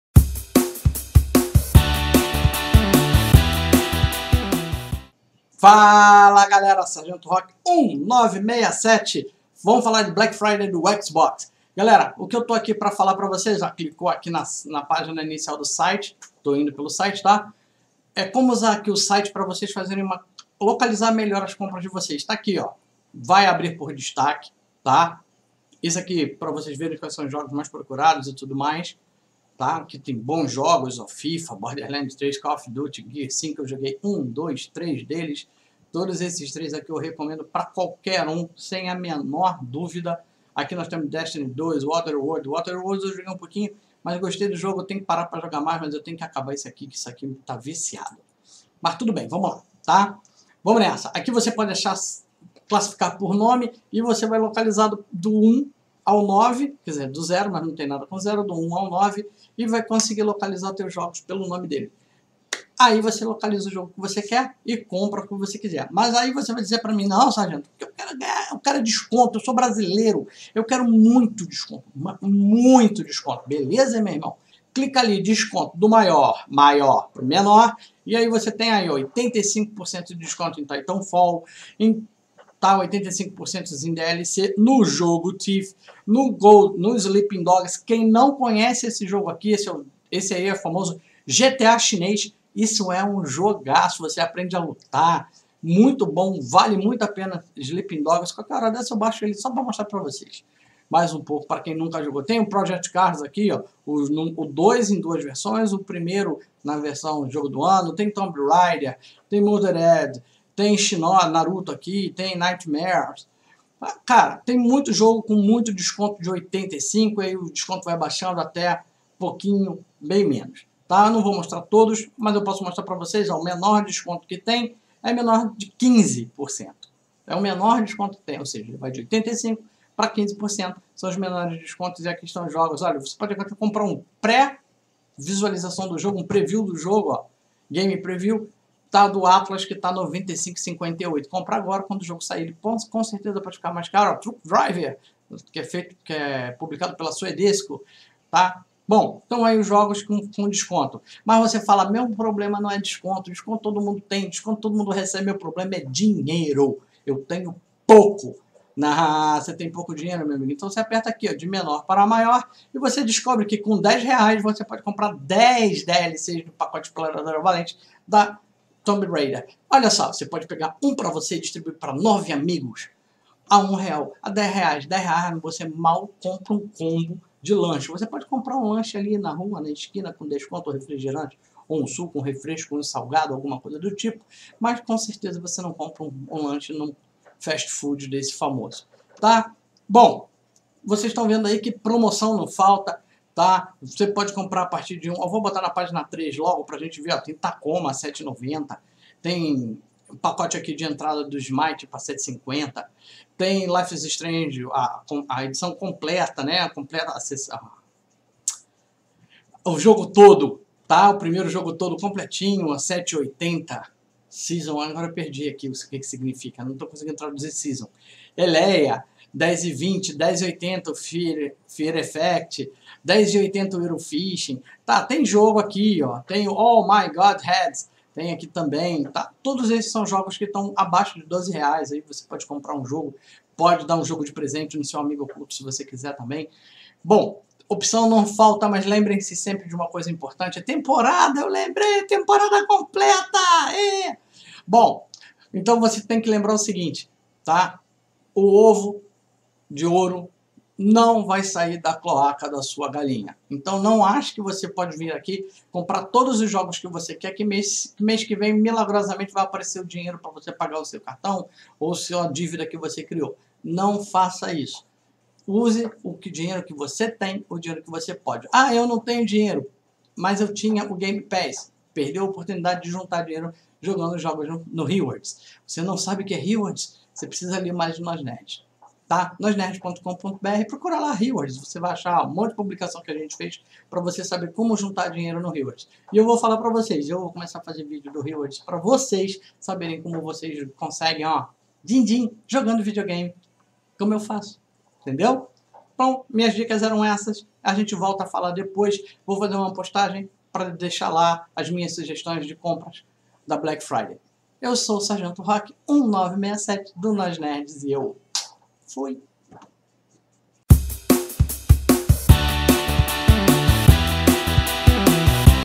Black Friday Xbox 2019 – Nós Nerds
Uma dica diferente sobre a Black Friday do Xbox! Vejam o vídeo e sejam felizes com a promoção!